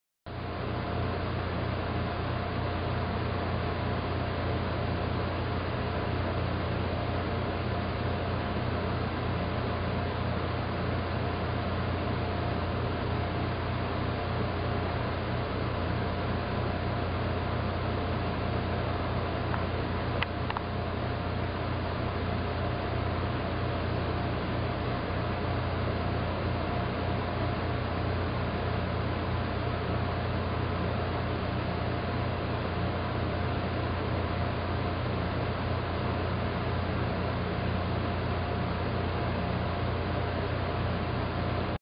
Archer & Hugo napping
Best buddies taking a much deserved nap after a long day of playing.  Look at Hugo kneading on Archer:)